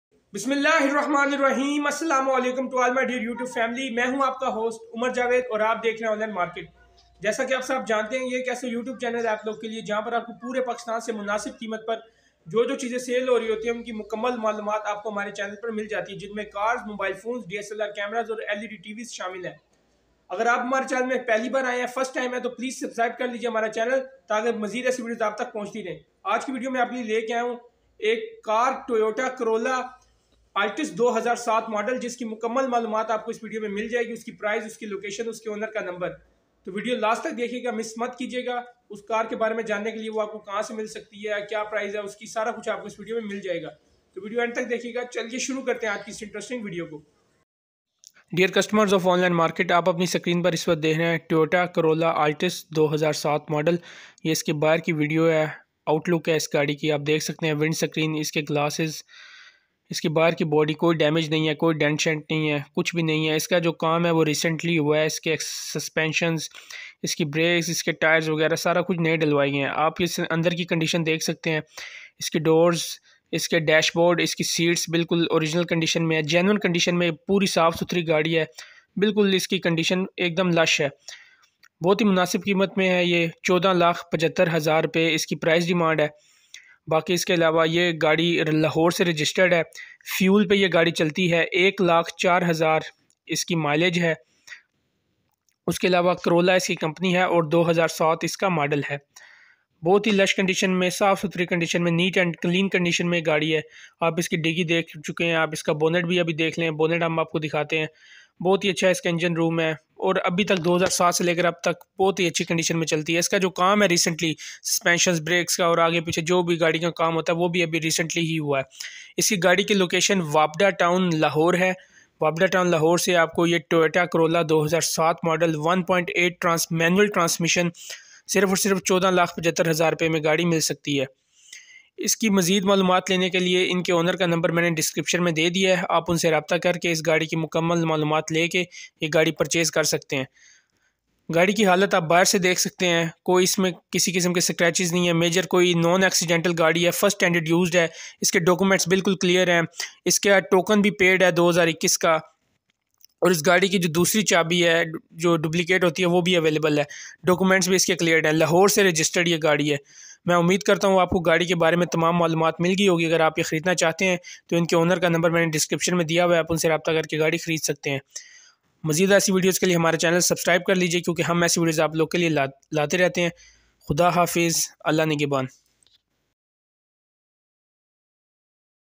Toyota Corolla Altis 2007 For Sale in Lahore || Toyota Corolla Altis 2007 Manual || Toyota 1.8
Toyota Corolla Altis 2007 For Sale in Lahore

Car Details

Toyota Corolla
2007 Model
1.8 Manual Transmission
Used
Consumes Petrol
104,000 Kms Driven (Mileage)
Lahore registered
Neat & Clean Condition
Rs.1,475,000 Price Demand
All Mechanical Work recently done Includes Suspensions, Shocks, Plugs and tuning
Warranty Book available

Owner Details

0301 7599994
Wapda Town, Lahore